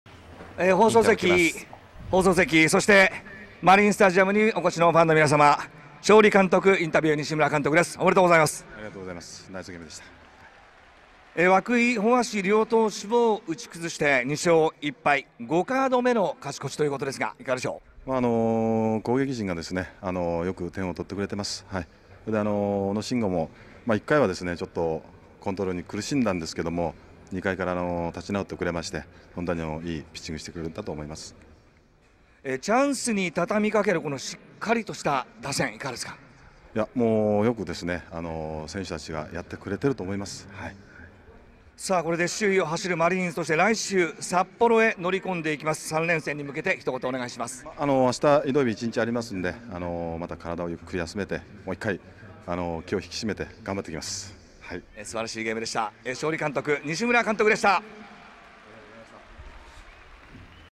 西村監督 勝利インタビュー 4月11日 ロッテ11-0西武
今季のカード勝ち越しが5カード目となり「攻撃陣がよく点を取ってくれている」と勝利インタビューに応える西村監督。 （4月11日 ロッテ-西武戦）